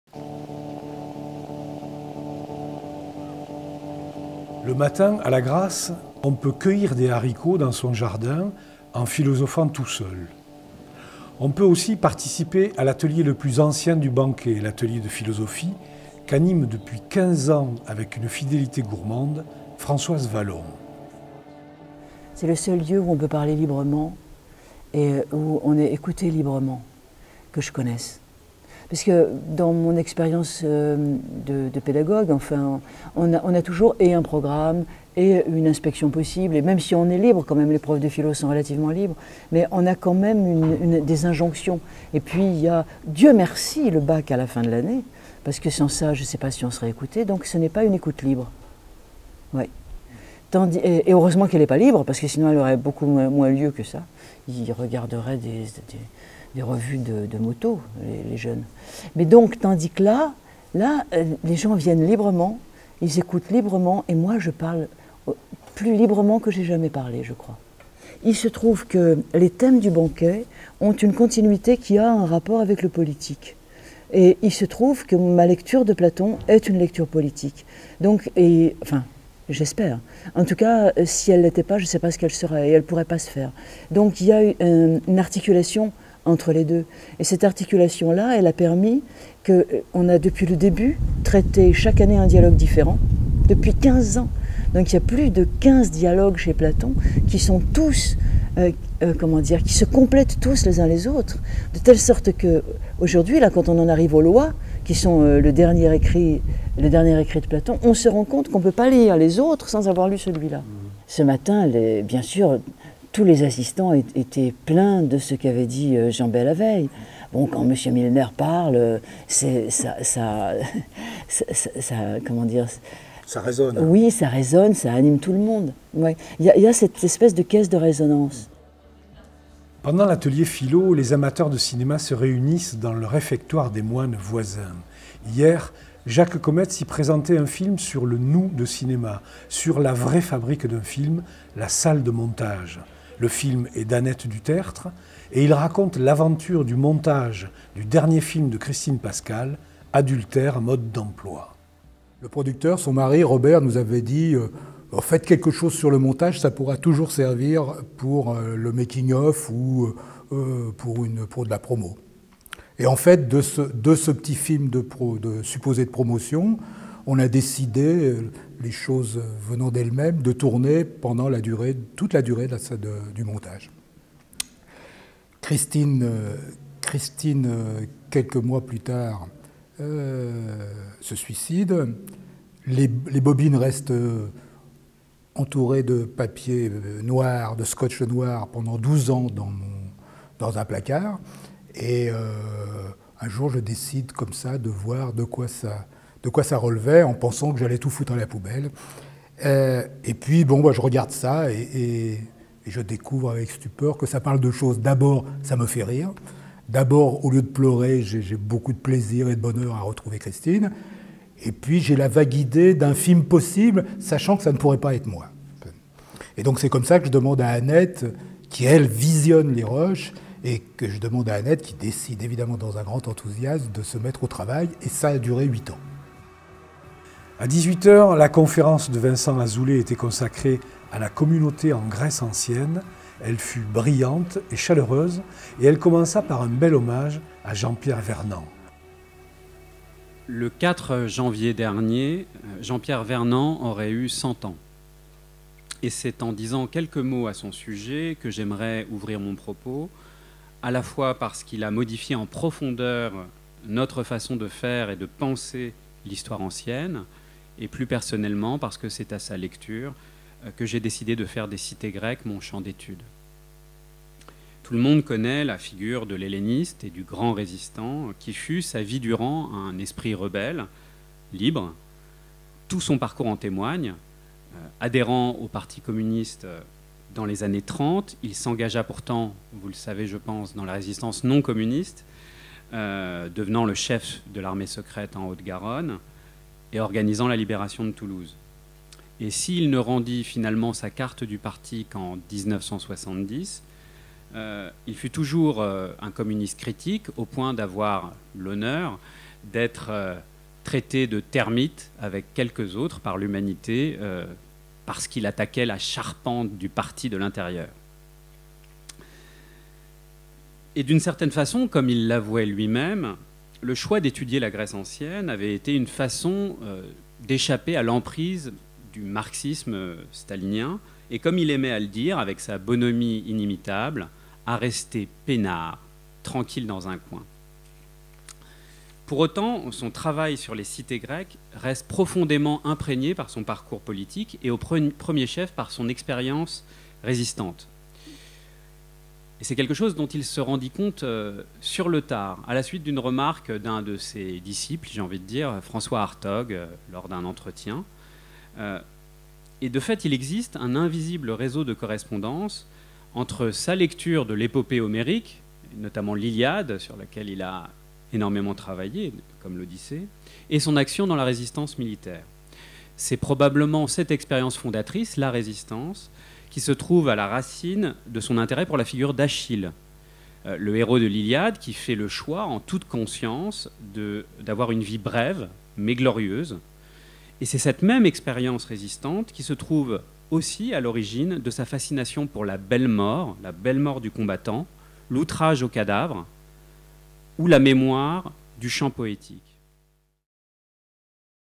Corbières Matin n°51
Cinquième numéro de cette série 2016 du quotidien vidéo du Banquet du Livre de Lagrasse. Mercredi 6 août 2014.